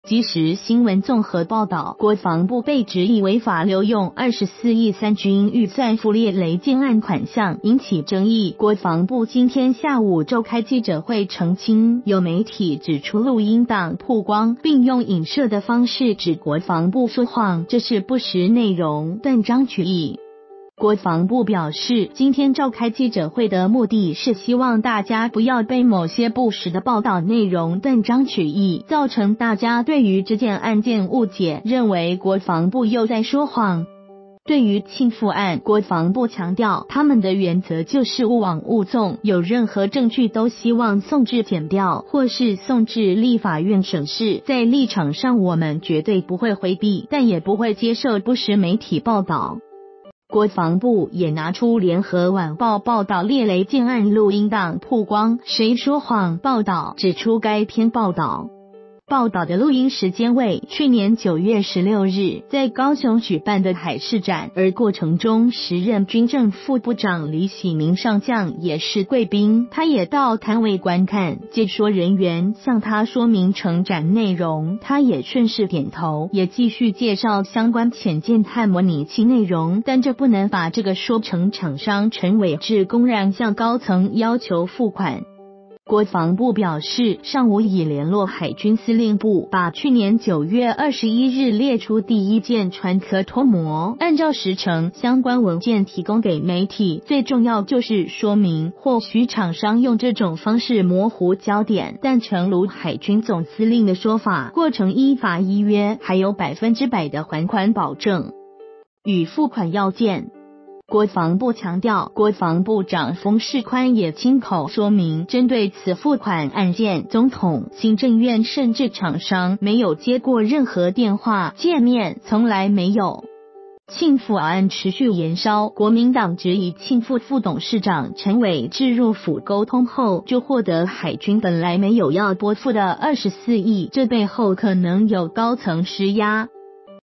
慶富案被質疑說謊 國防部批媒體報導不實、斷章取義
〔即時新聞／綜合報導〕國防部被質疑違法流用24億三軍預算付獵雷艦案款項，引起爭議，國防部今天下午召開記者會澄清，有媒體指出錄音檔曝光，並用影射的方式指國防部說謊，這是不實內容、斷章取義。